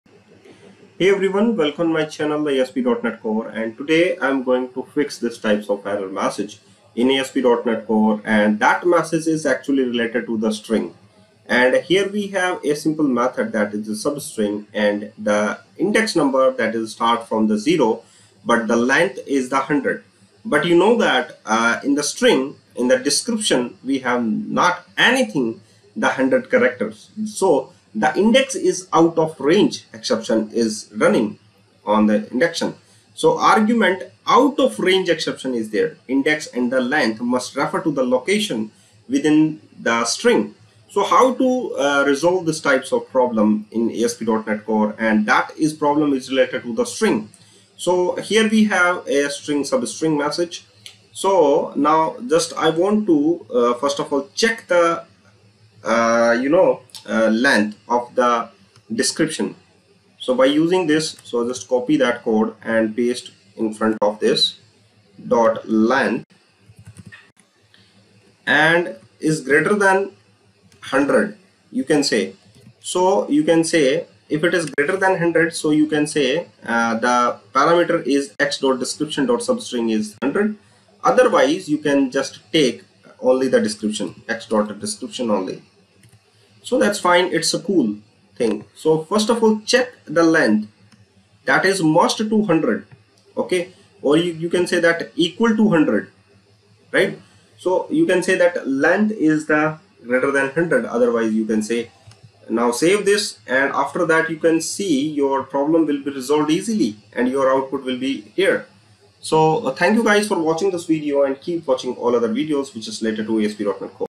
Fix: Index and length must refer to a location within the string | SubString | C# | ASP.NET CORE
How to Fix : this error in c# , which is related to string
AgrumentOutOfRangeException : Index and length must refer to a location within the string